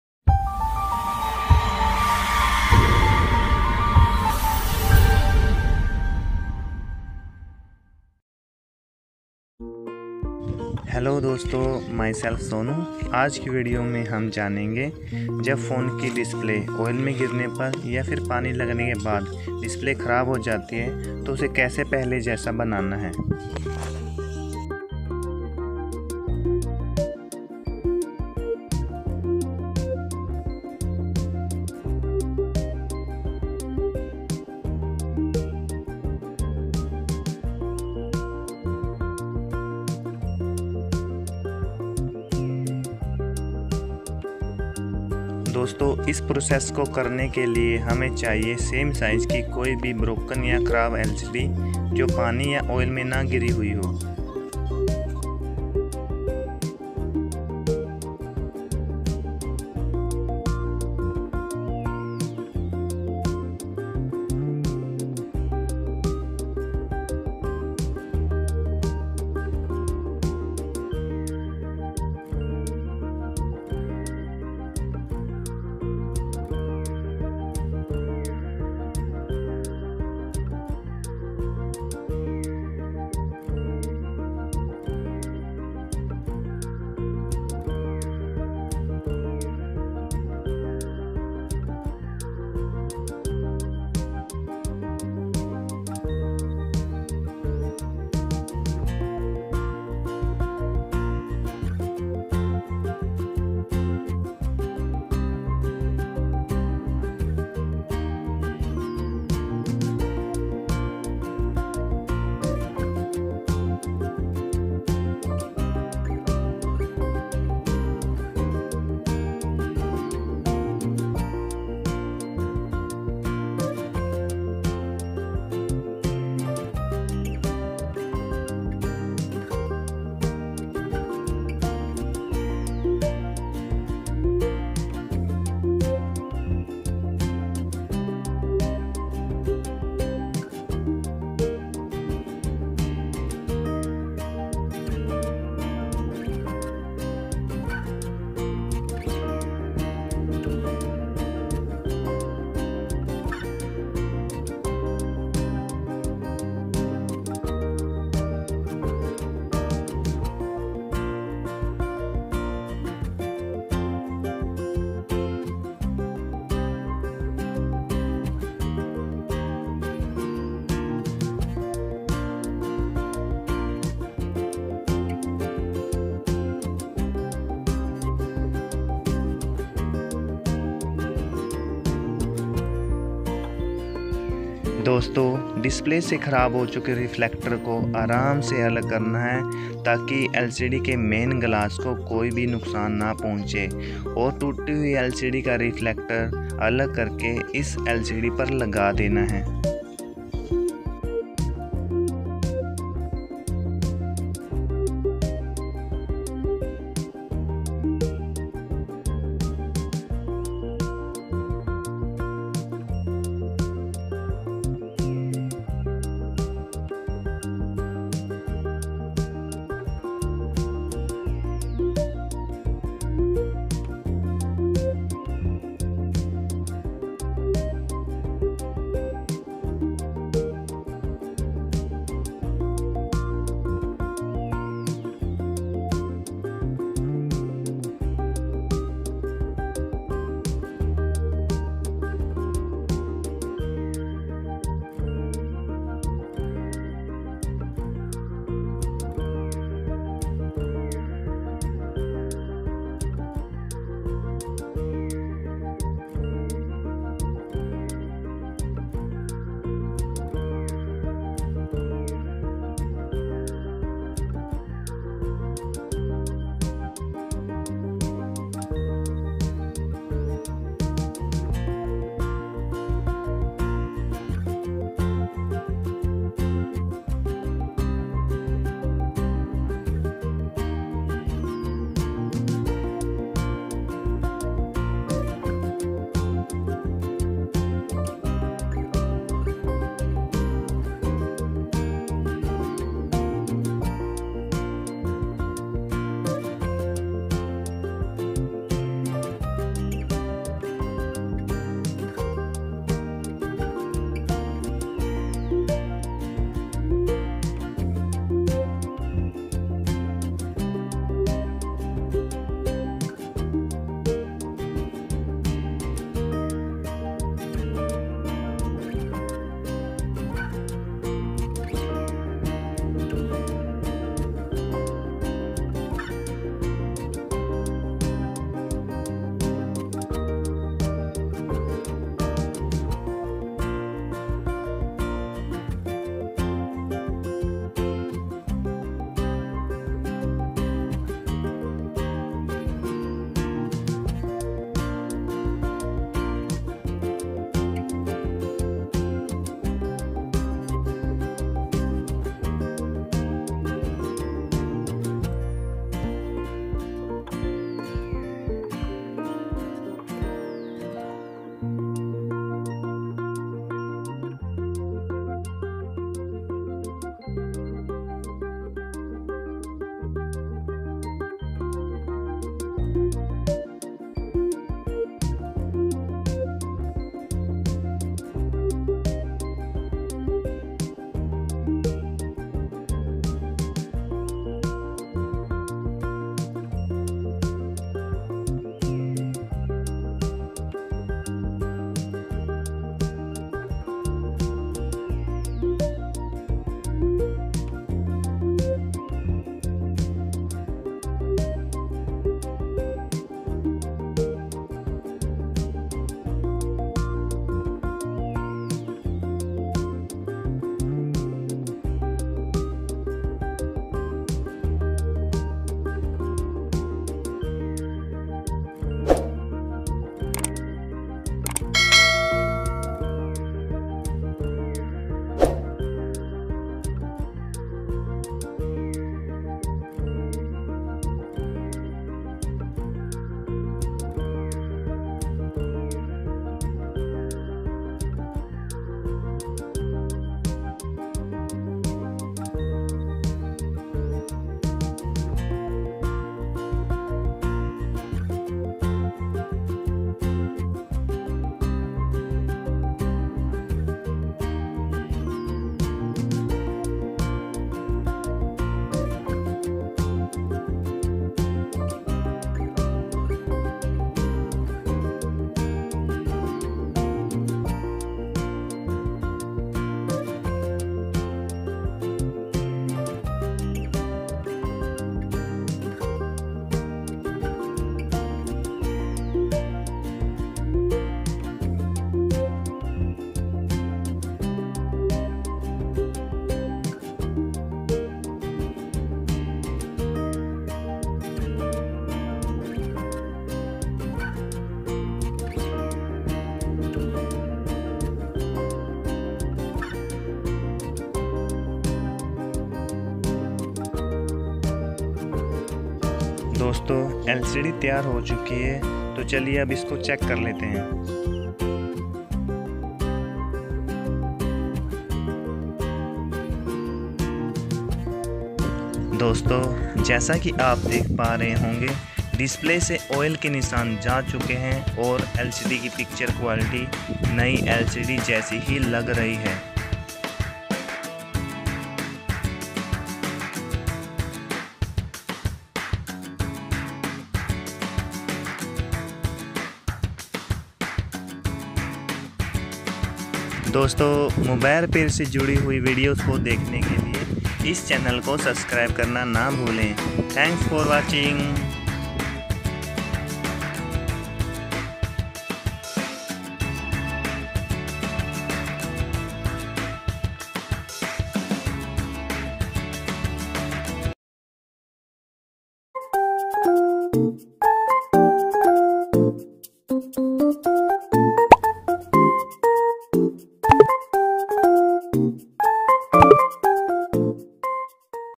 How to fix Oil and water Damage Display
# Oil & water#Damage LCD

Is video mein aap dekhnge Jab phone Ki LCD mein Oil ya fir Pani lagne pr LCD mein Dots aa jate hai to unko
kese theek krna hai .